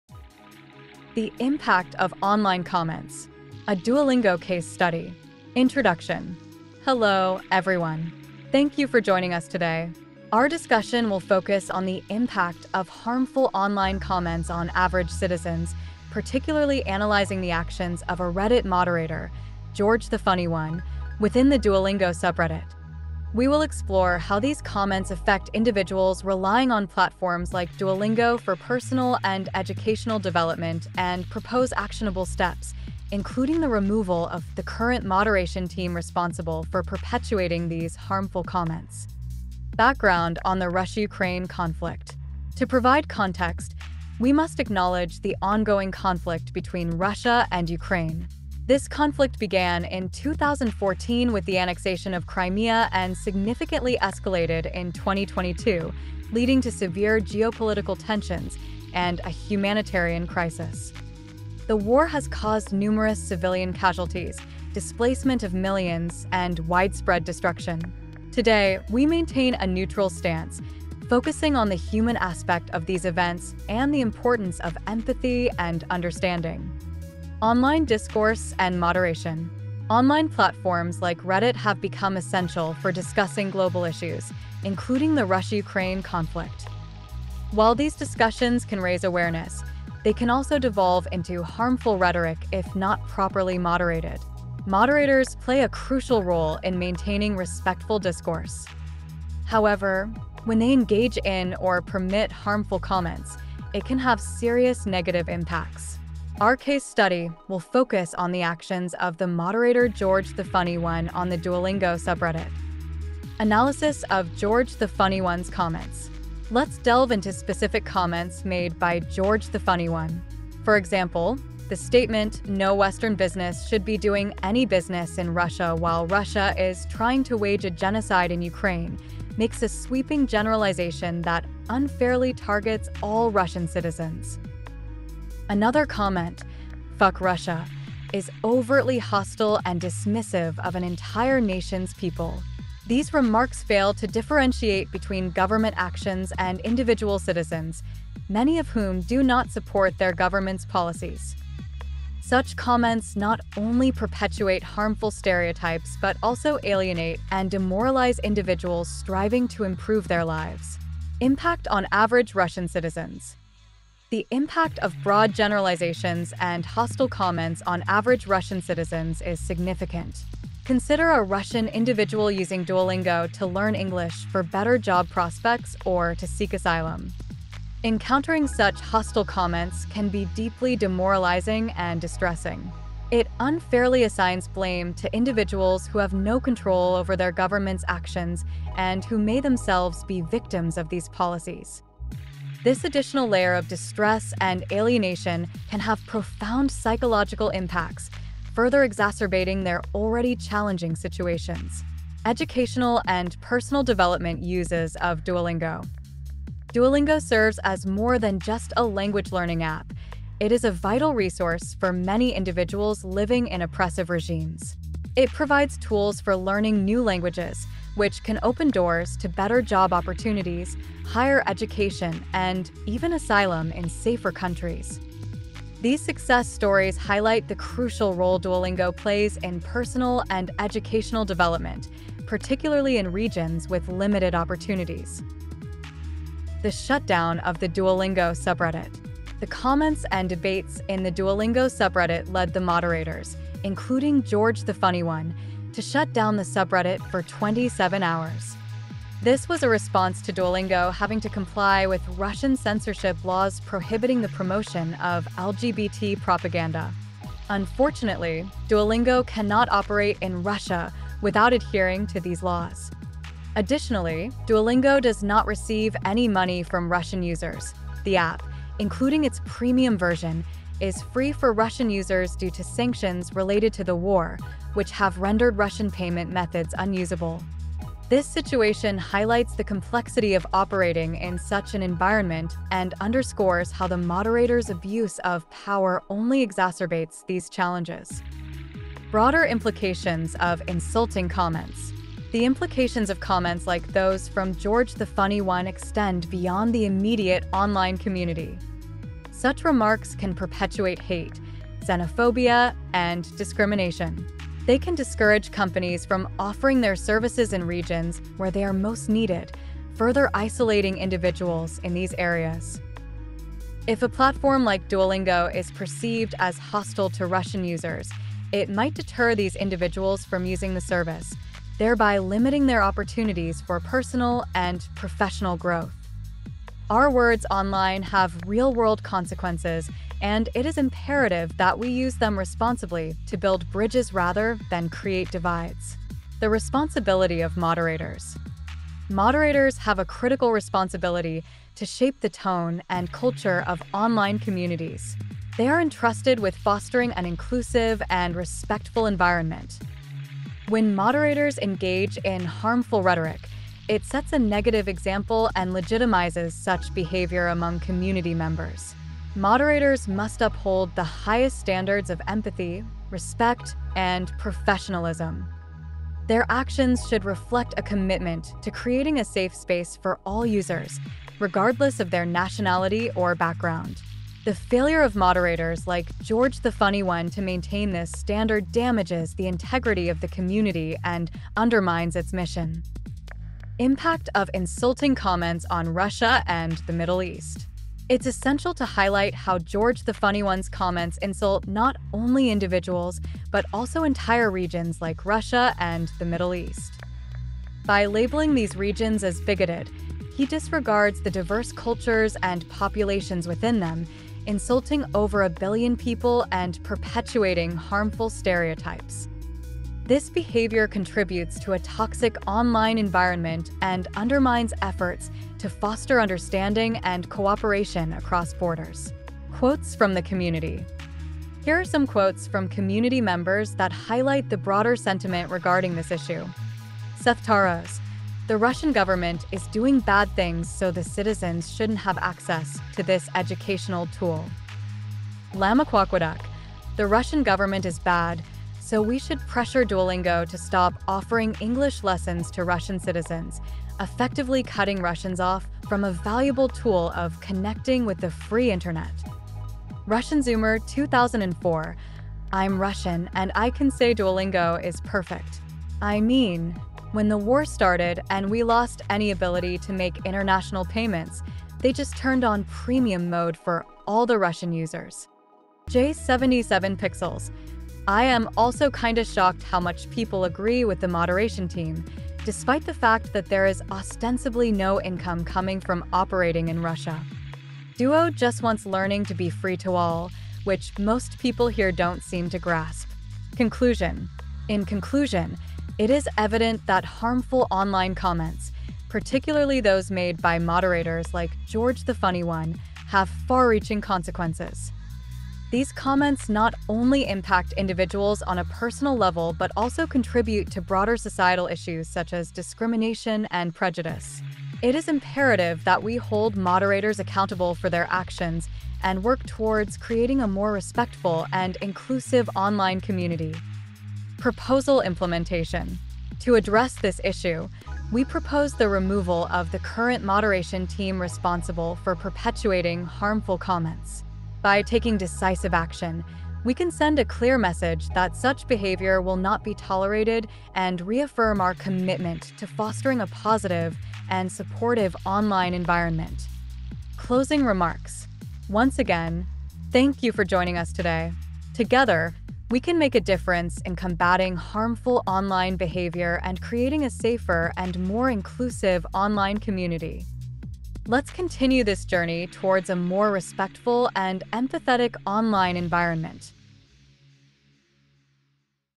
Duolingo Promoting Stereotypes & Violence on Reddit
Hello, everyone. Today, we'll examine the impact of harmful online comments, focusing on Reddit moderator GeorgeTheFunnyOne from the Duolingo subreddit. His hostile remarks, particularly against Russians, highlight how online discourse can perpetuate stereotypes and negatively affect individuals seeking personal and educational growth through platforms like Duolingo. These comments, including derogatory statements about Russia and the Middle East, insult over a billion people, perpetuating xenophobia and discrimination. As online communities play a crucial role in global conversations, moderators must maintain respectful environments. This case underscores the need for accountability and a push towards more inclusive and supportive online spaces. We propose removing the current moderation team to reaffirm this commitment. Together, we can foster a more empathetic online environment.